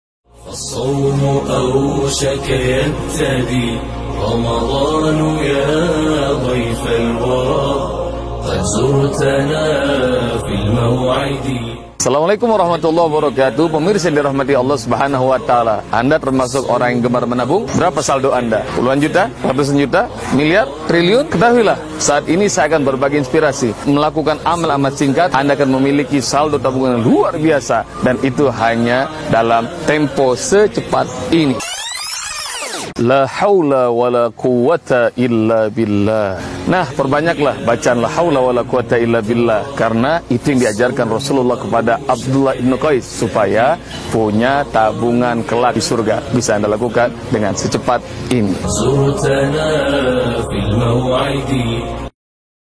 Secepat Ini - Laaulawalaquwwata Illa Billah
Menabung harta di bank itu sudah biasa, namum bisakah Anda menabung harta di surga, tanpa harus mengeluarkan banyak uang. Amatlah mudah dan hanya SecepatIni bagi umat Islam, Anda hanya perlu duduk manis dan tidak sampai satu menit maka sudah tersimpan harta terbaik Anda di surga, apakah itu?

Ya cukup berucap لاَ حَوْلَ وَلاَ قُوَّةَ إِلاَّ بِاللَّهِ kalimat yang mudah diucap ini akan menjadi tabungan istimewa di surga

Mulai dari sekarang, kita rutinkan amalan hebat ini yang membuat Anda mempunyai tabungan di surga.